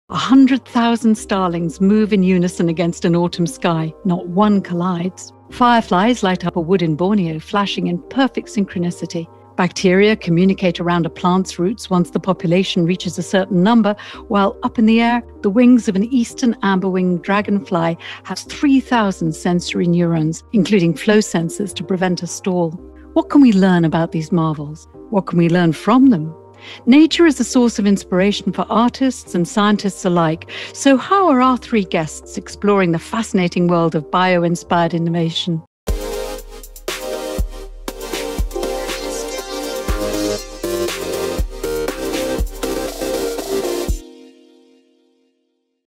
Bio-inspired Innovation
In this CORDIScovery episode,  we are looking at the evolving world of bio-inspired innovation: the analysis and systematic transfer of biological insights into technical applications. Our guests are all exploring biological solutions at the interface between living nature and technology.